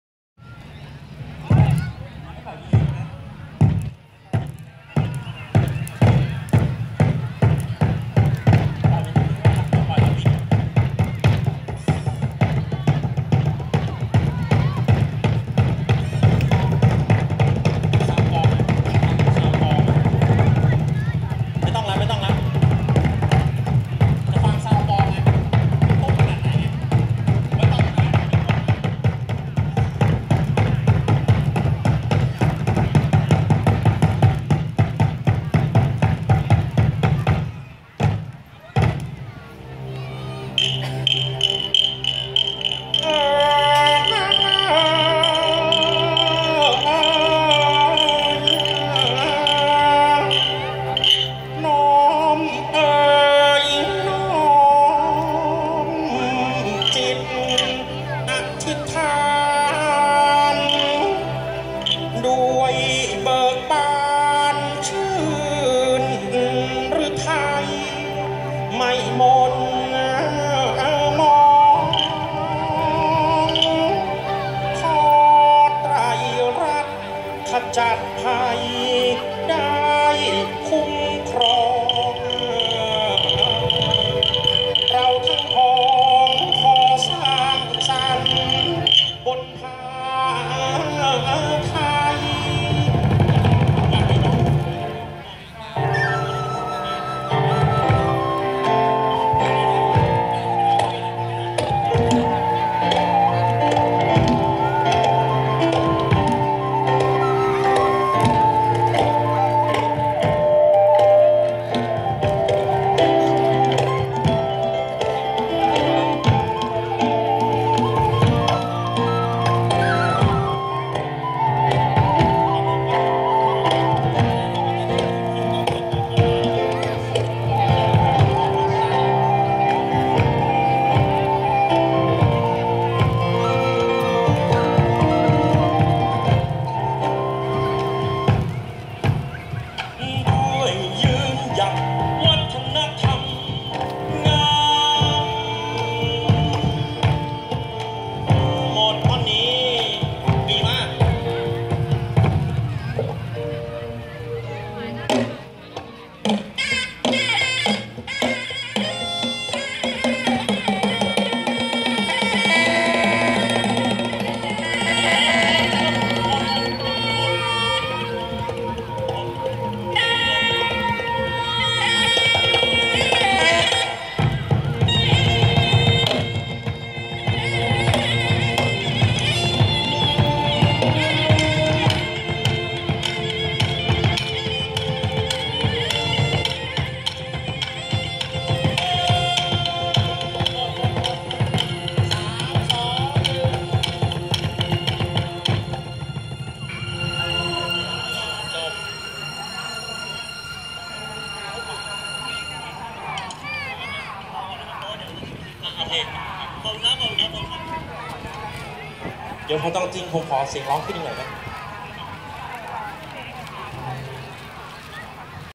ซ้อมขับเสภาเทพๆ ณ สนามหลวง
24 เมษายน 2559  คุณนพพร เพริศแพร้ว
ใต้ร่มพระบารมี 234  ปี กรุงรัตนโกสินทร์ ณ ท้องสนามหลวง